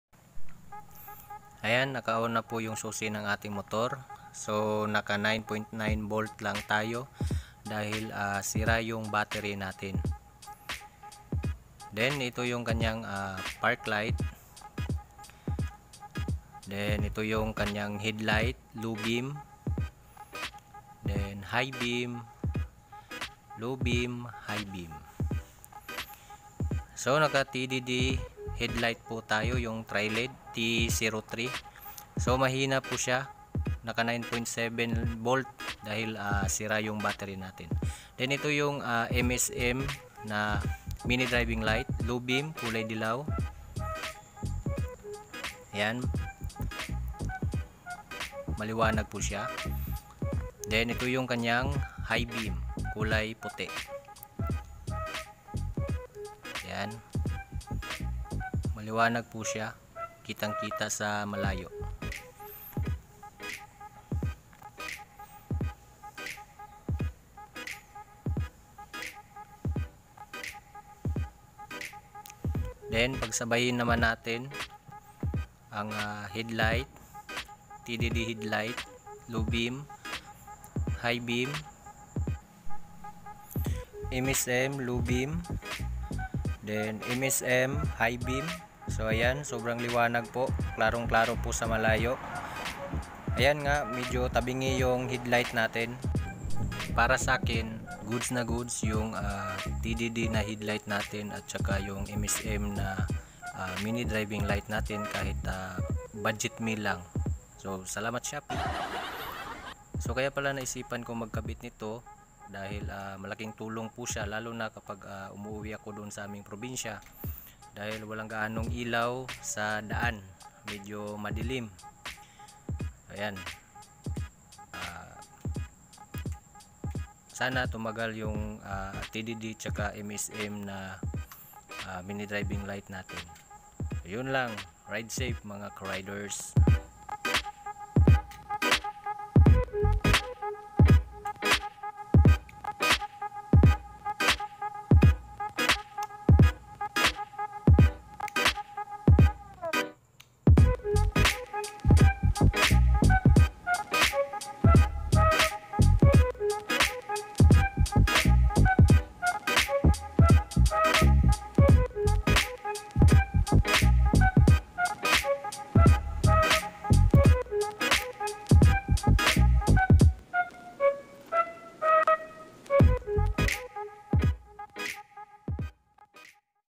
TDD HEADLIGHT AND MSM MINI DRIVING LIGHT BUDGET MEAL NA ILAW NG MOTOR
TDD HEAD LIGHT AND MSM MINI DRIVING LIGHT BUDGET MEAL SET UP NA ILAW NG MOTOR, MALAKAS DIN NAMAN SWAK NA SWAK SA KANYANG MURANG PRESYO ANG TANONG IS KUNG TATAGAL BA PERO I HOPE SANA TUMAGAL HEHE, NEXT IS PALIT BATTERY NA NAMAN KAPAG SOBRANG SIRA NA TALAGA BATTERY NG MOTOR KO.